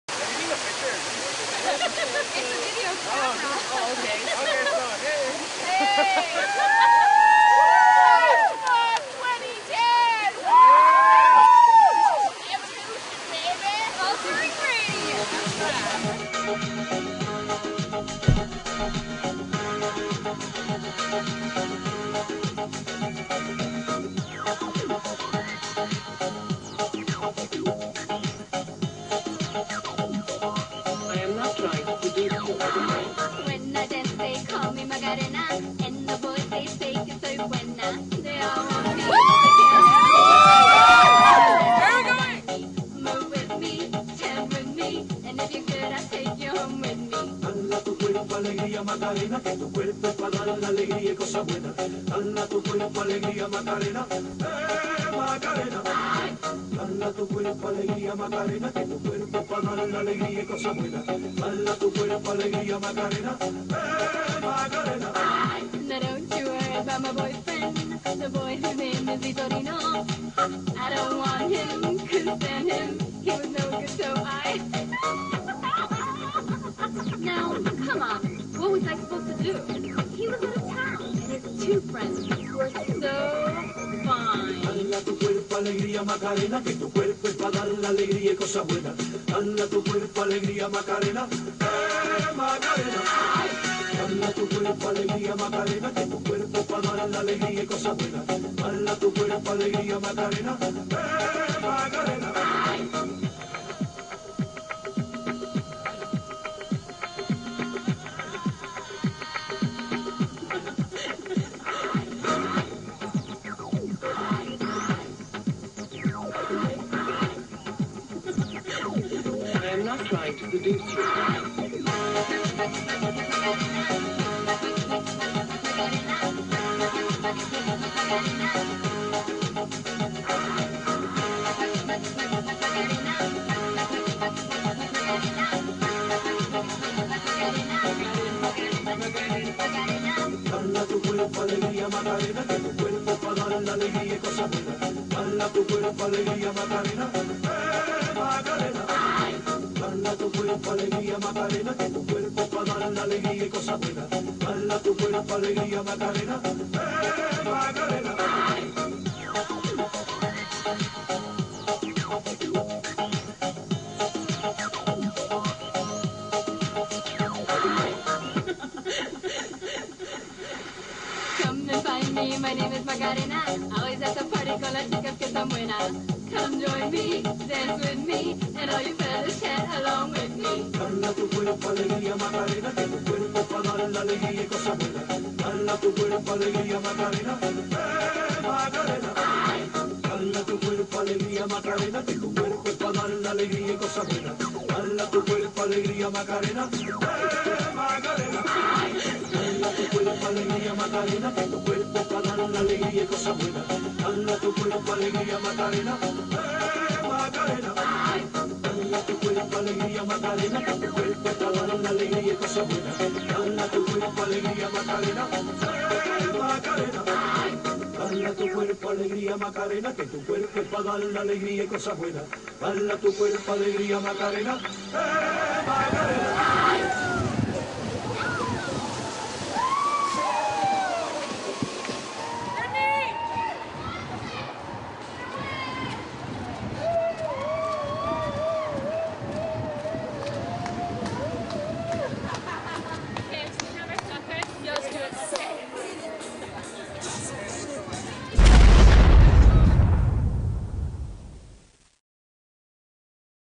Evolution 107.9 Flash Mob
BCIT's Evolution 107.9 radio station organized a Flash Mob at the Art Gallery in Downtown Vancouver on March 10, 2010